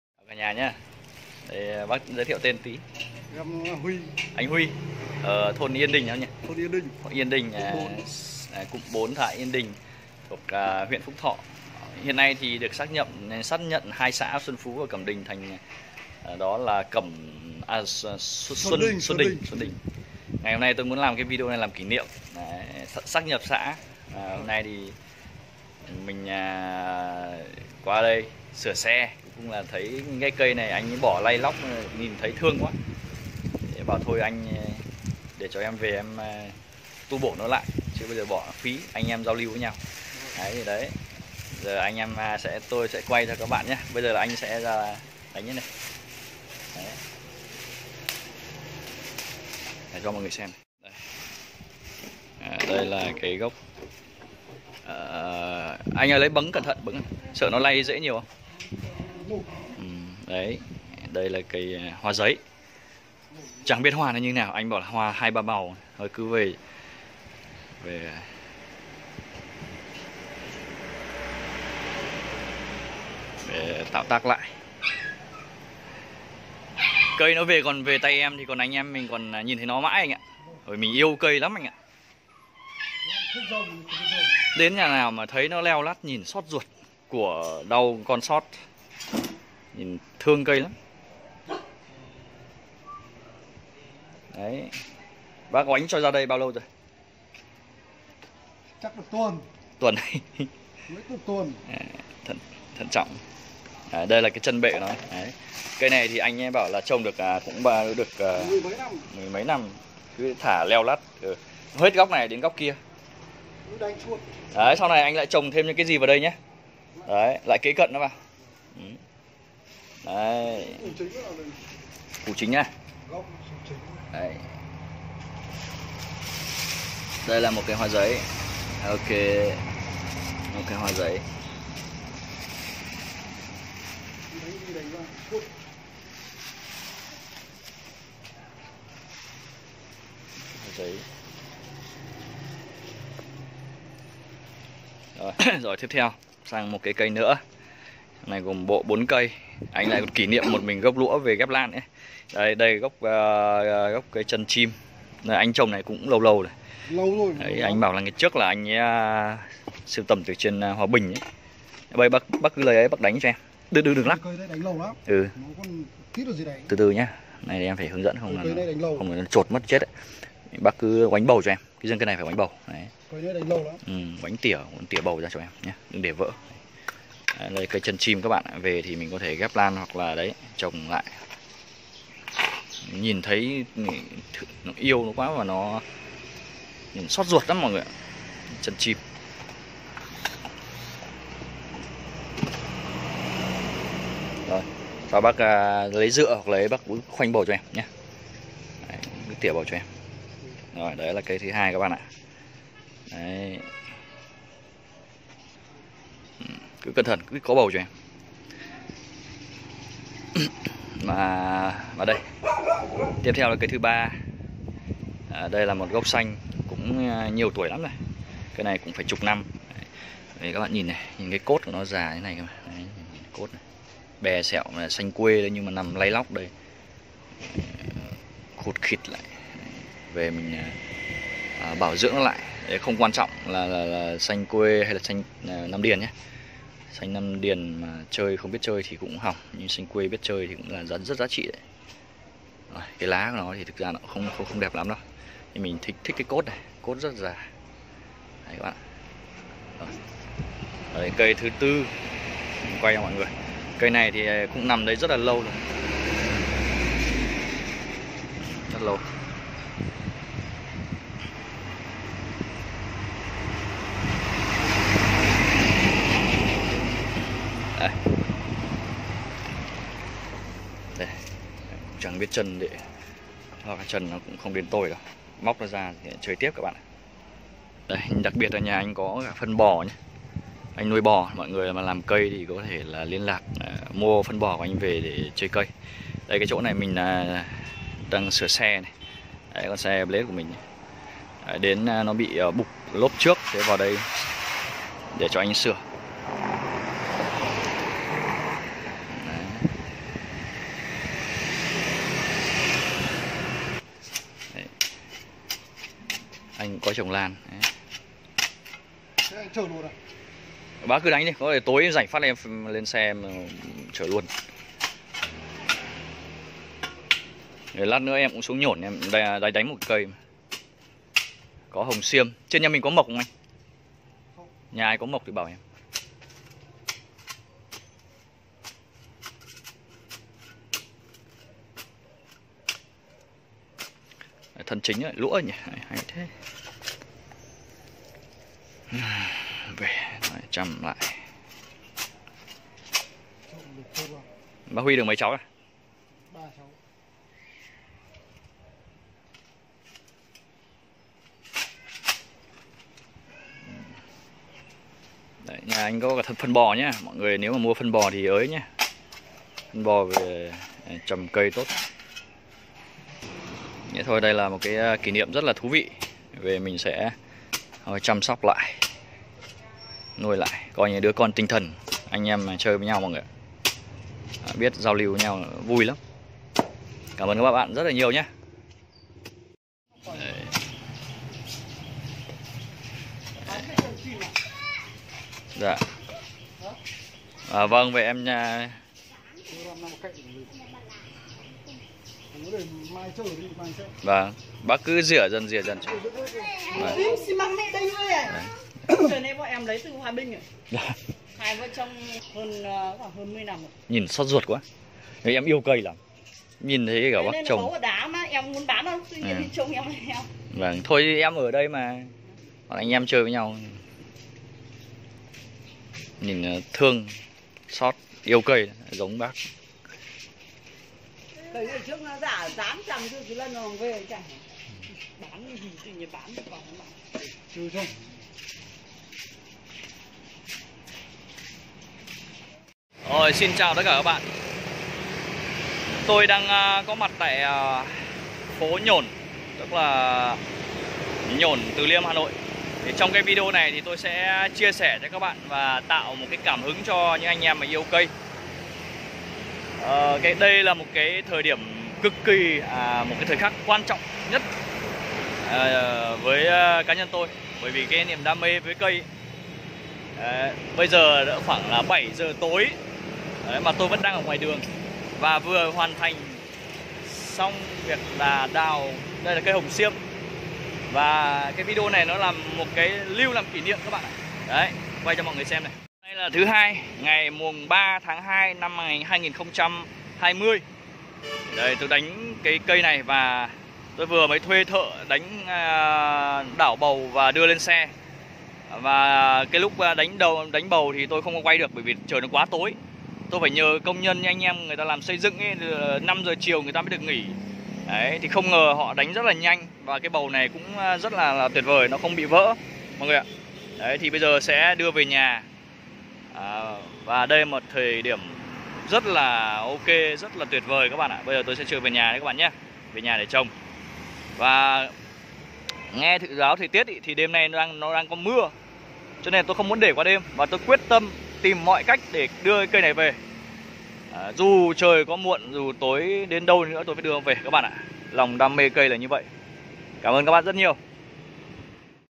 ĐI SĂN CÂY NGƯỜI KHÁC BỎ VỀ CHƠI LẠI
Đi mua cây của dân về chăm lại